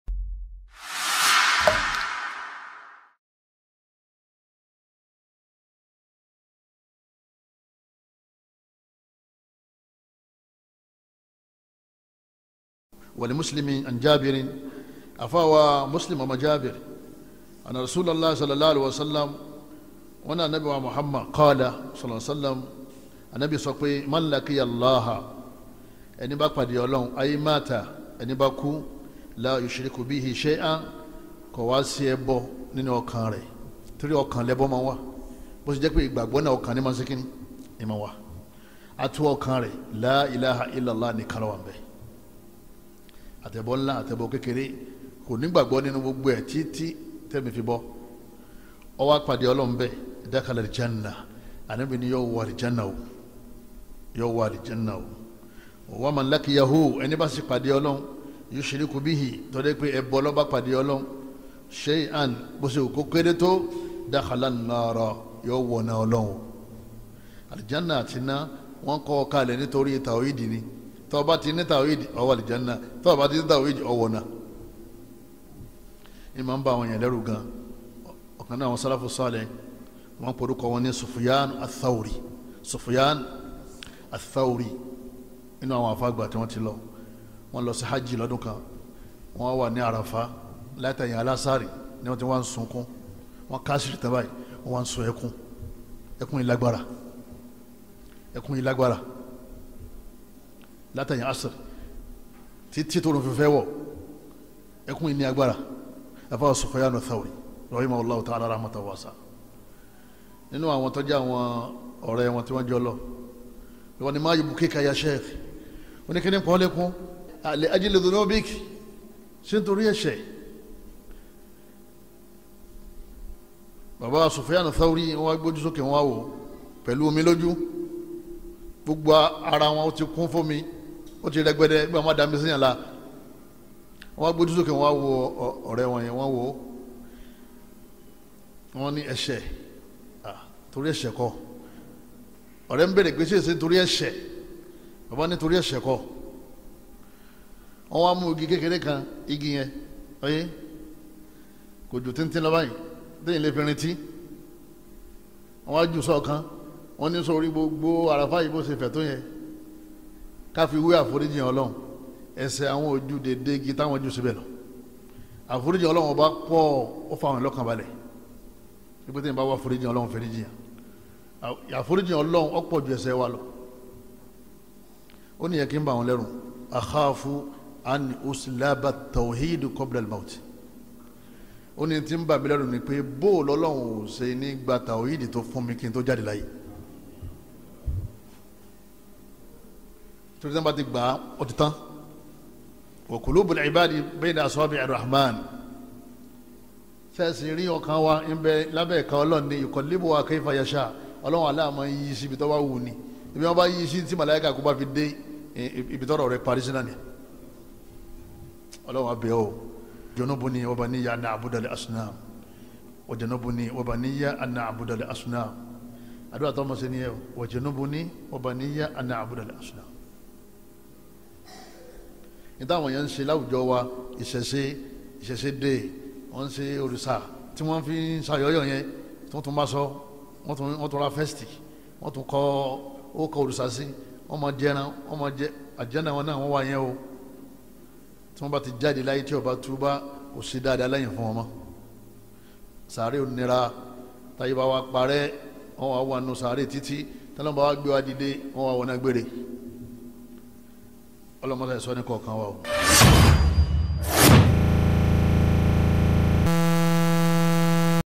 THE REWARD OF DIEING UPON TAW'HEED By: Fadilatul Sheikh Qomaruden Yunus Akorede hafizohulloh
Daaru's Salam tv